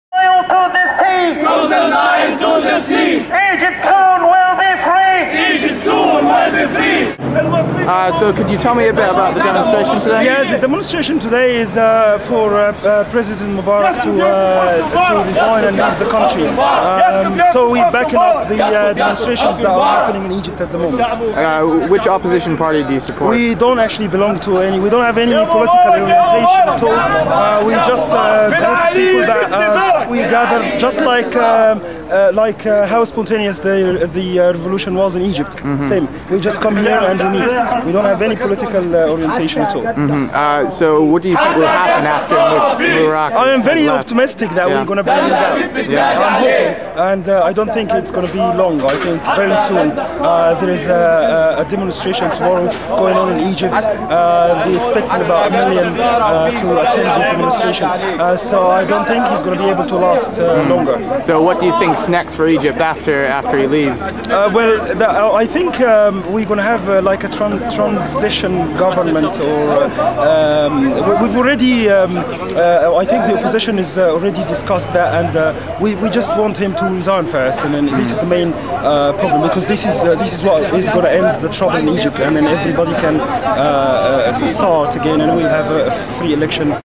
Protest at Egyptian Embassy London - Jan 31 2011
Joe Stohlman covers the protest at the Egyptian embassy for Think Africa Press.